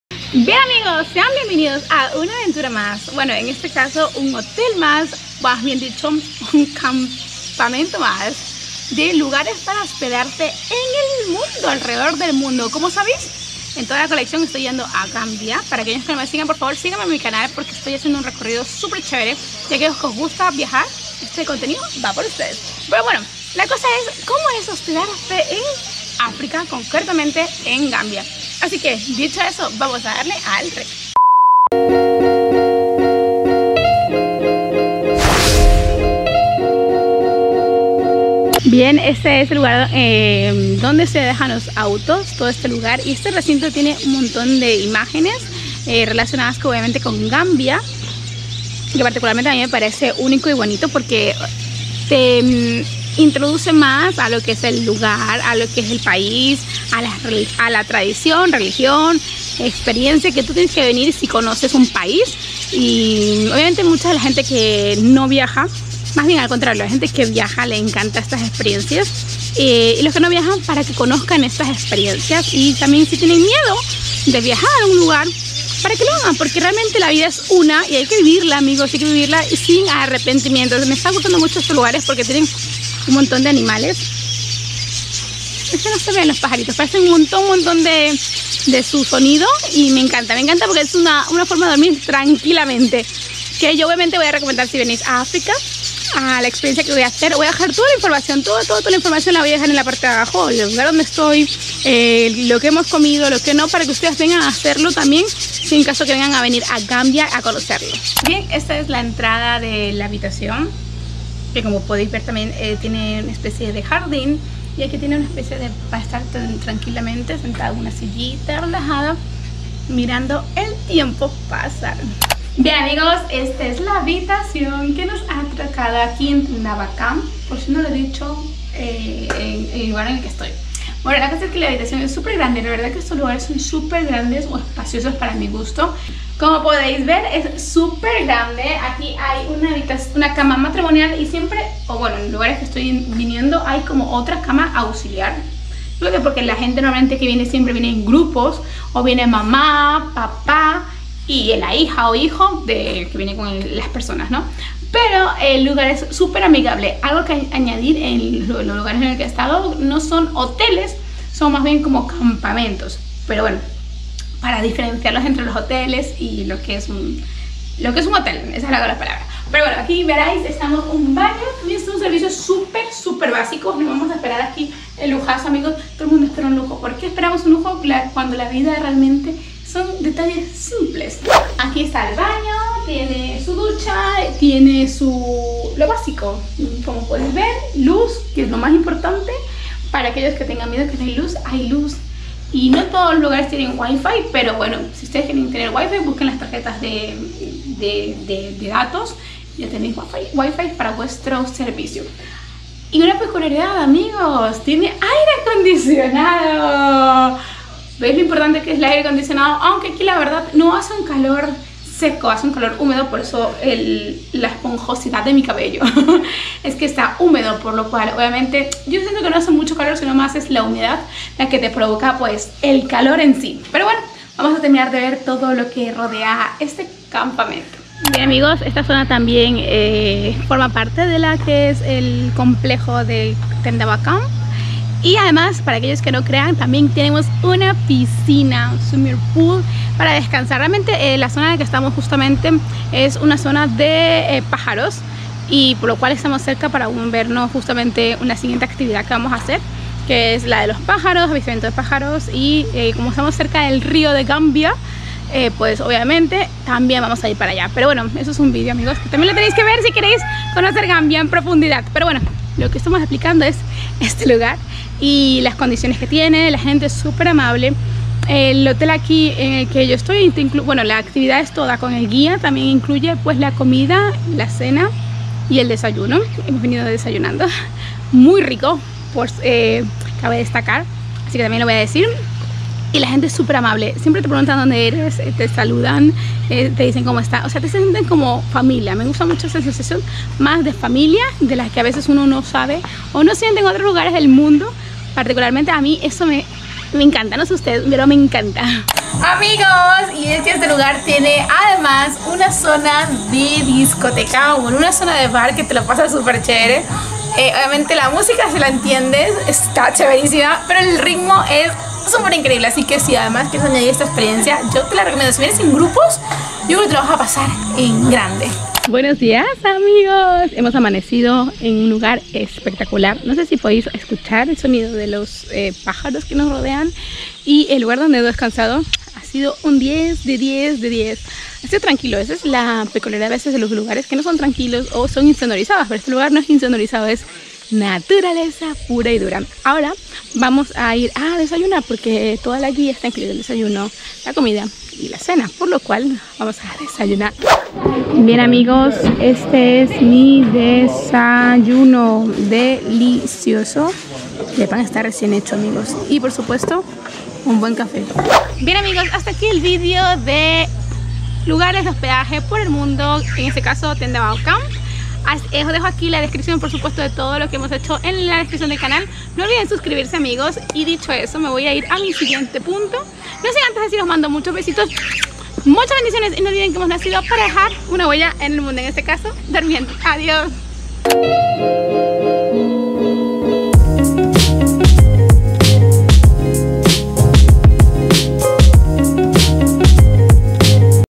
ASÍ es HOSPEDARSE 🛏💤en un Campamento en Tendaba | Gambia🇬🇲
🛌💤Amig@s me van acompañar a conocer habitaciones del mundo de toda clase, en esta ocasión Tendaba Camp en Gambia.
Si quieres una alternativa para hospedarte en Gambia, que te salga barata este es tu hospedaje. La mejor idea es hospedarte en Gambia para quién quiere ahorrarse dinero y vivir una experiencia nueva.

Feliz día!! 😊😊

Y no olvides que hemos nacido para dejar una huella en el mundo👣👣👣👣👣👣👣👣

¿QUIERES SER MI AMIG@? 😊😊